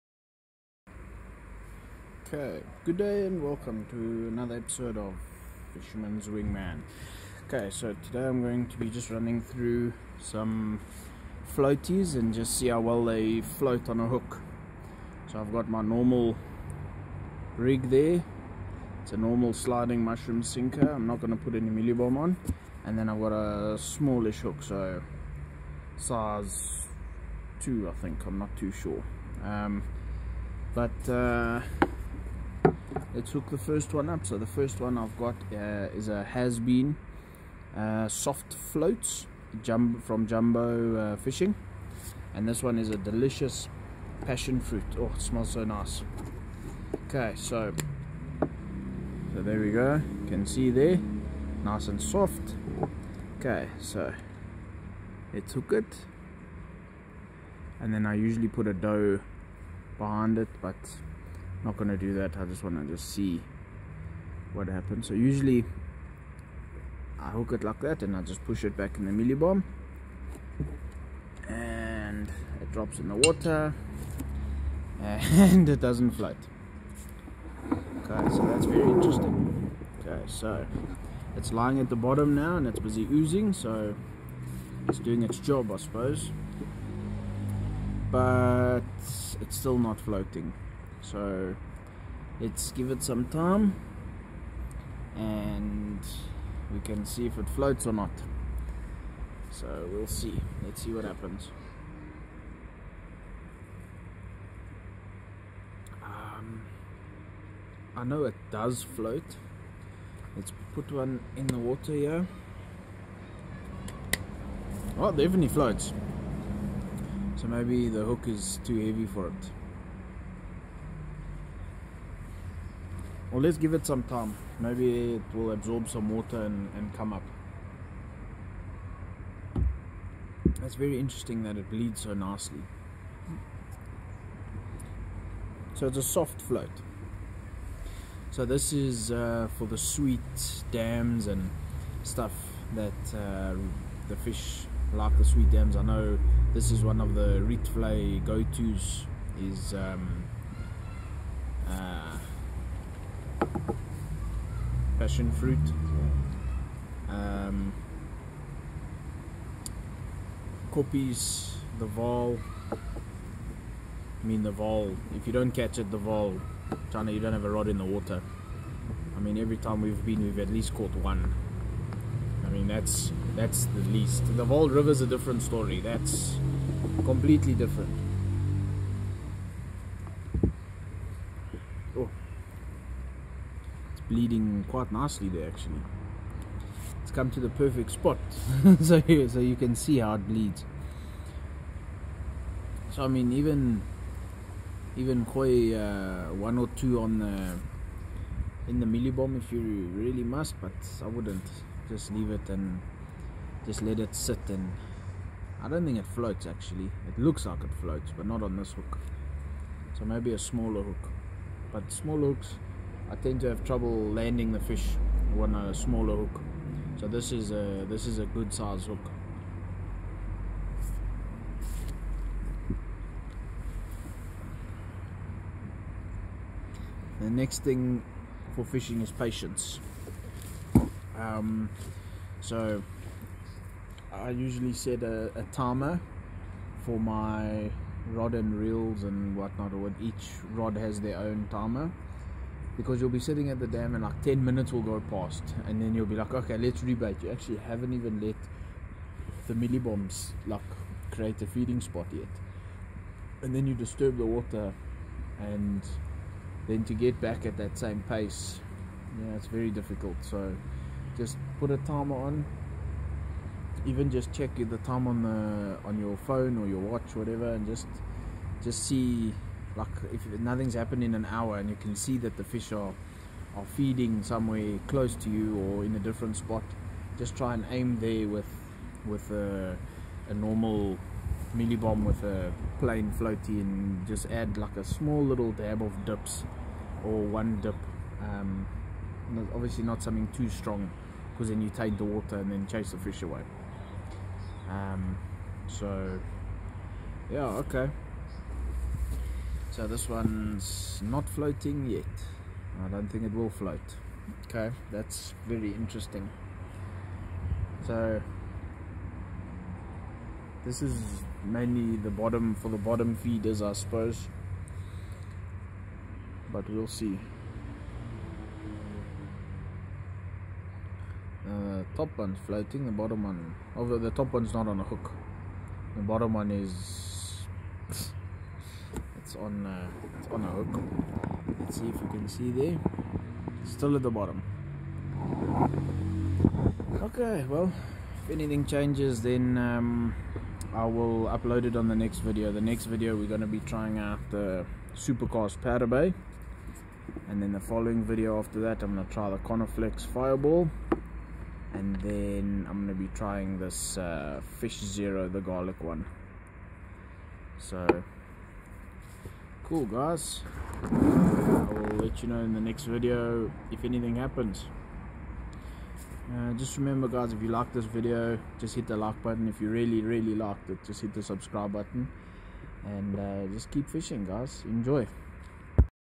Jumbo Soft Float Test
Today we try out Jumbo's Has Been Soft Float and see what happens under the water... Will it float? Find out